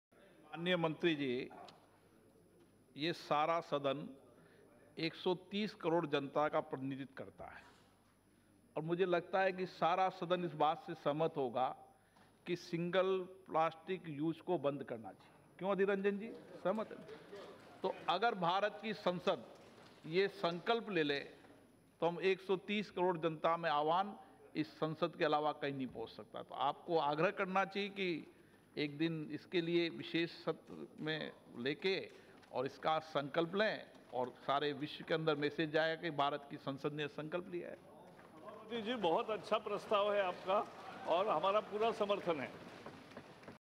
Resolution against Single Use Plastic- 22/11/2019
HS Appeal of bring Resolution in Parliament against Single Use Plastic